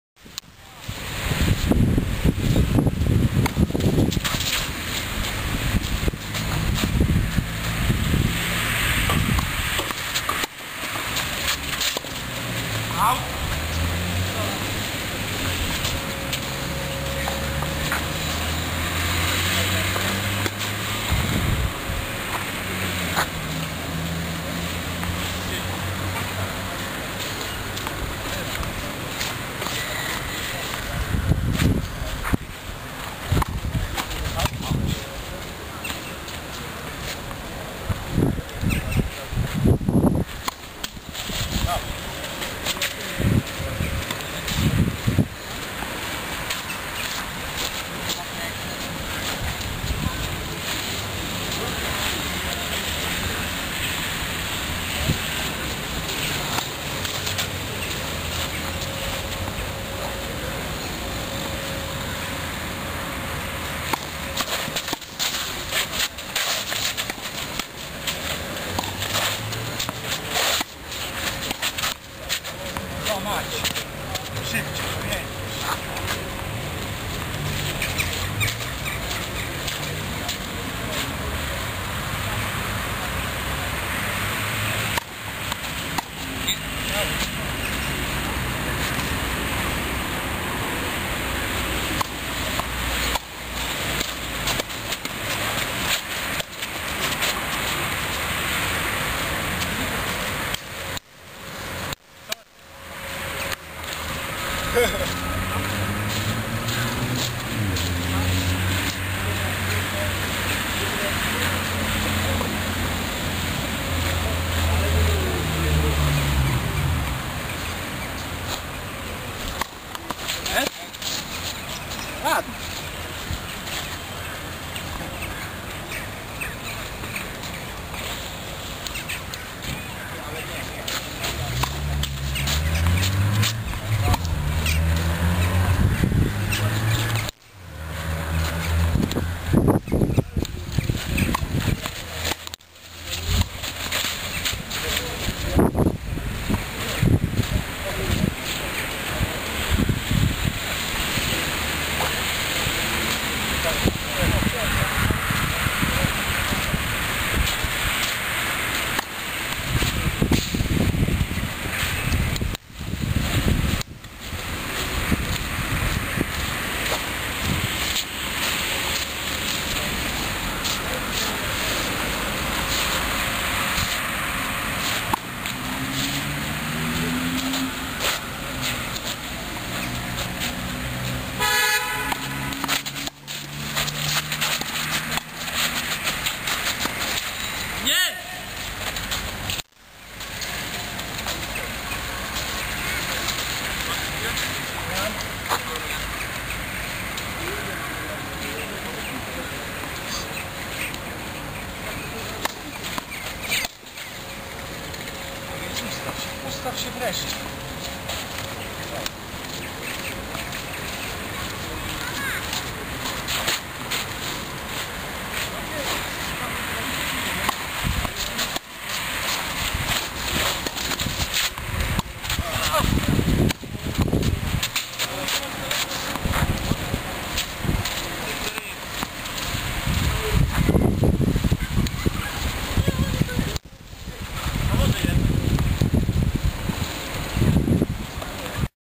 Deblowe Mistrzostwa Ostrołęki
Mecz Otłowski/Nosek - Wyrębek/Mrozek w ramach Deblowych Mistrzostw Miasta Ostrołęka.